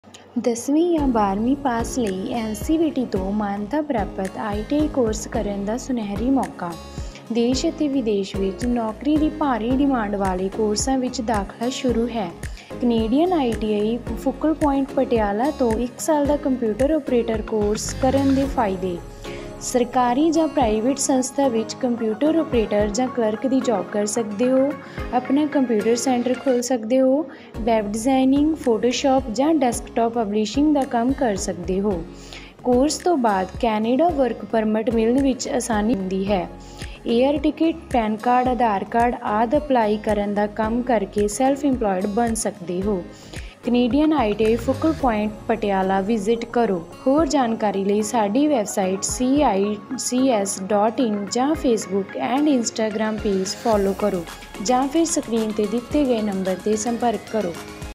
ITI ਵਿੱਚ 1 ਸਾਲ ਦਾ ਕੰਪਿਊਟਰ ਕੋਰਸ (COPA) ਕਰਨ ਦੇ ਫਾਇਦੇ
Canadian ITI Patiala ਵਿੱਚ 1 ਸਾਲ ਦਾ ਕੰਪਿਊਟਰ ਕੋਰਸ (COPA) ਕਰਨ ਦੇ ਫਾਇਦੇ
ਦੇਸ਼-ਵਿਦੇਸ਼ ਵਿੱਚ ਸਰਕਾਰੀ ਅਤੇ ਪ੍ਰਾਈਵੇਟ ਨੌਕਰੀ ਲਈ ਜਰੂਰੀ, NCVT ਭਾਰਤ ਸਰਕਾਰ ਤੋਂ ਮਾਨਤਾ ਪ੍ਰਾਪਤ ਕੋਰਸ |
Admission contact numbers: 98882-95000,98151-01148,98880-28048